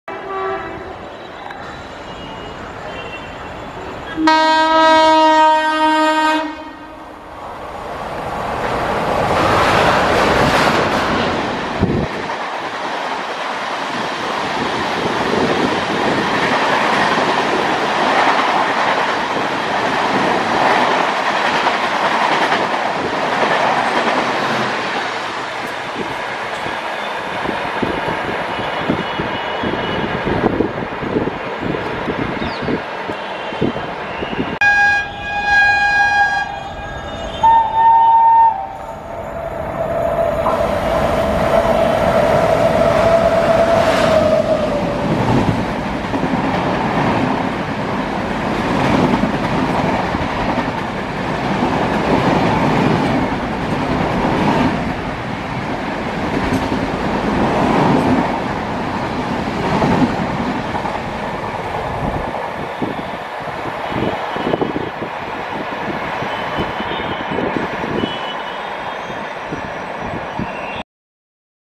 Deccan Queen & Mumbai Howrah Duronto Back to back High Speed Mps Action At Dombivli
Back To Back High Speed Action

12123 Mumbai Pune Deccan Queen Express Hauled By Kalyan WCAM-3 &

12261 Mumbai Howrah AC Duronto Express Hauled By SRC WAP7

Furiously Skips Dombivli At Their MPS